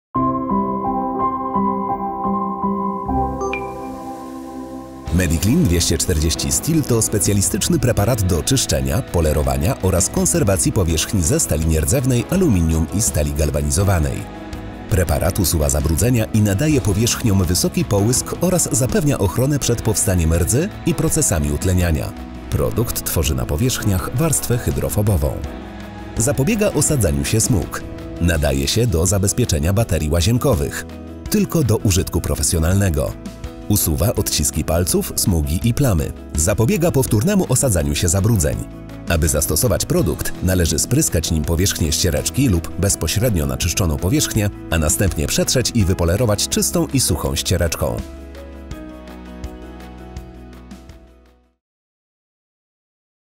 Mediclean 240 Steel
Preparat do czyszczenia i pielęgnacji powierzchni ze stali nierdzewnej.

Gotowy do użycia preparat przeznaczony do czyszczenia, polerowania oraz konserwacji powierzchni ze stali nierdzewnej, aluminium i stali galwanizowanej. Usuwa zabrudzenia i nadaje powierzchniom wysoki połysk oraz zapewnia ochronę przed powstaniem rdzy i procesami oksydacji (utleniania). Usuwa odciski palców, smugi i plamy. Zapobiega powtórnemu osadzaniu się zabrudzeń. Tylko do użytku profesjonalnego.